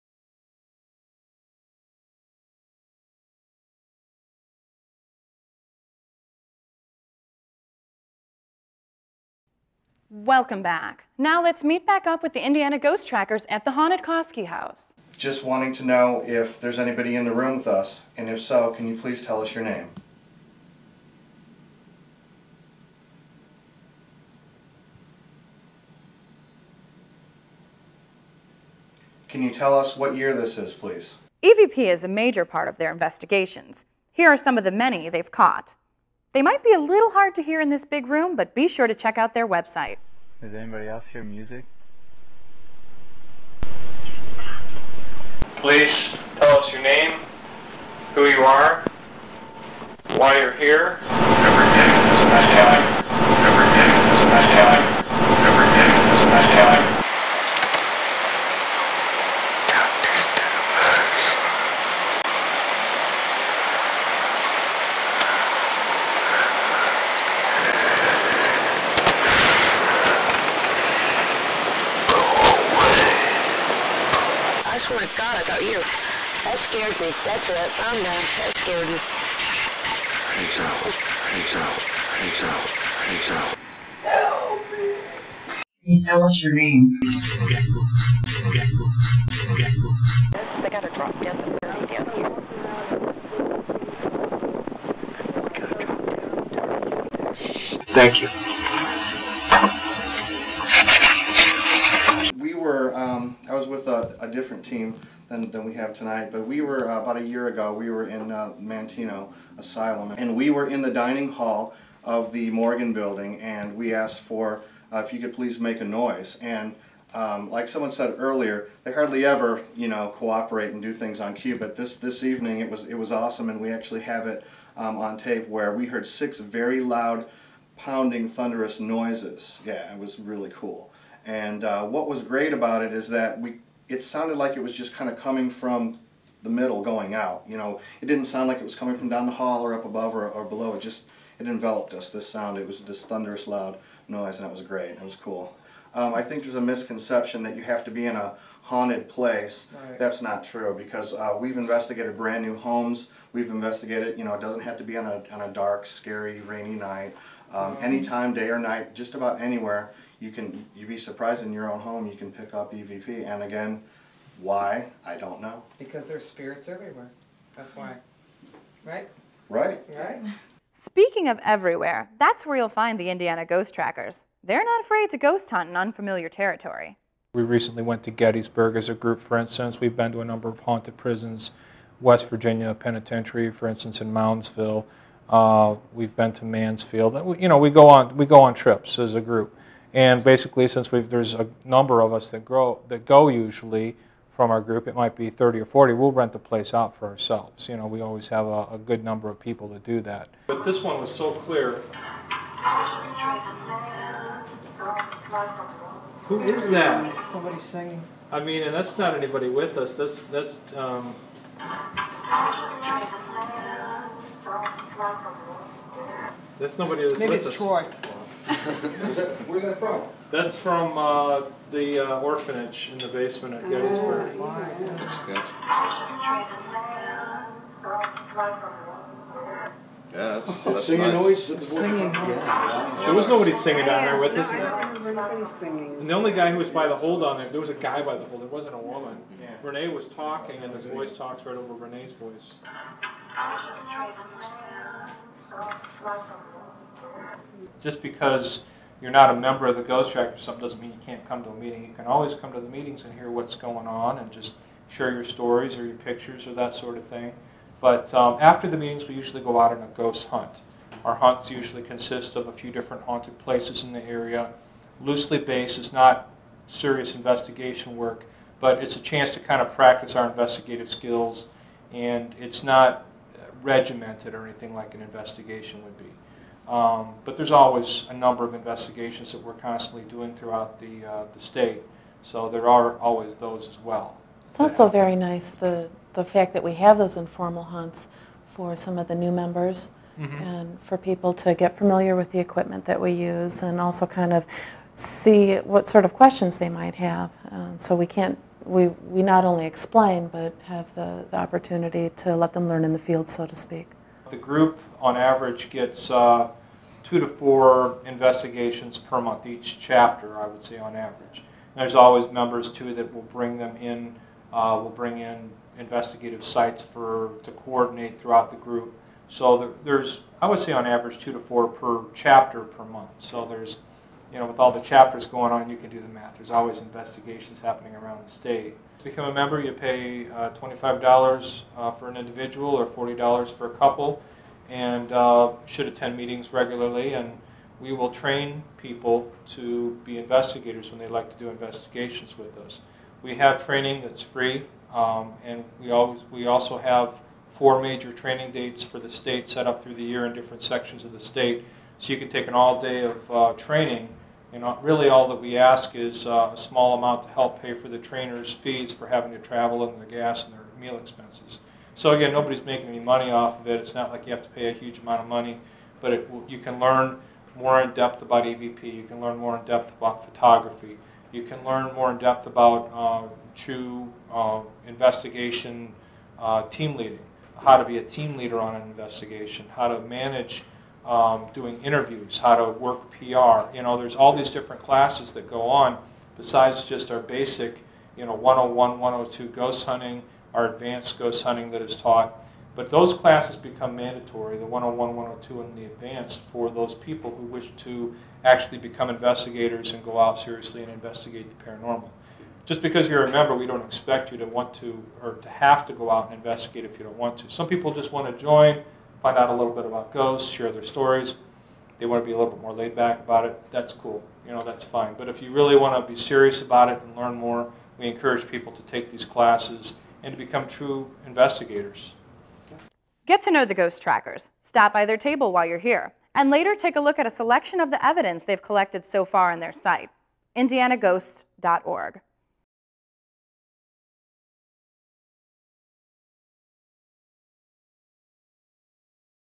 INDIANA GHOST TRACKERS PART 2
Part 1 only seen at dailymotion.com (due to YouTube's limitations (video length and file size)

We continue with the Indiana Ghost Tracker's at Munster Indiana's haunted Kaskie House.

Produced, written & Edited by Scott Markus
Voice over by Mary Czerwinski
Shot by Mary Gutfleisch
Robert Otterson - Grip